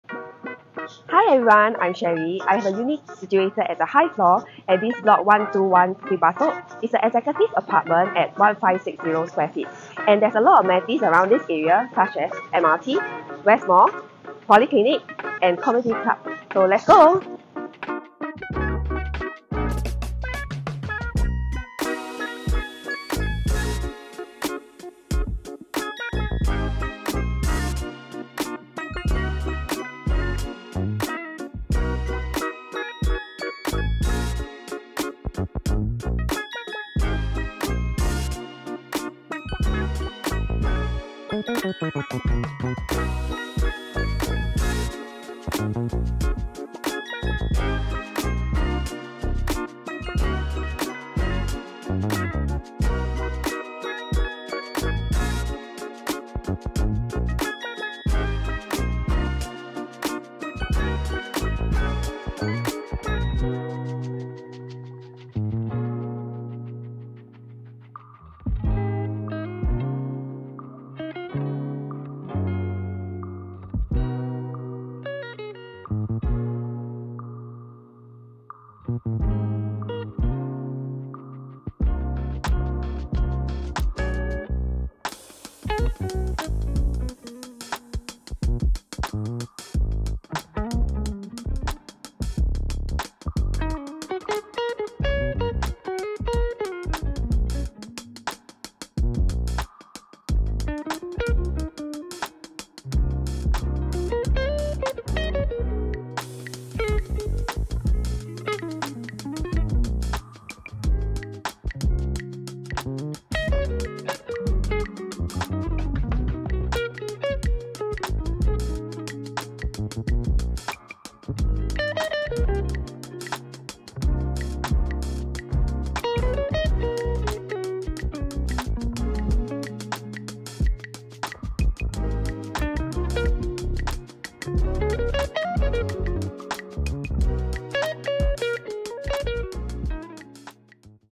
(Sold) 121 Bukit Batok EA for sale!
No Extension Required!

High floor, x2 spacious living area!
4 bed rooms in the unit - utility convert into room
Original condition
Additional recess area

Amenities
walkable distance to
✔️Bukit Batok Polyclinic
✔️Bukit Batok Community centre
✔️ Bukit Batok Central Park

5 mins away from
✔️ West Mall
✔️ Bukit Batok mrt
✔️ Bukit Batok interchange
✔️ Bukit Batok hawker

Schools
1km away from
✔️princess Elizabeth
✔️Dazhong pri
✔️Bukit view pri
✔️Lian hua pri

View to appreciate the spacious space!

Call / WhatsApp Sherry @ 8781. to arrange for an exclusive viewing!

Sherry goh
PropNex Realty Pte Ltd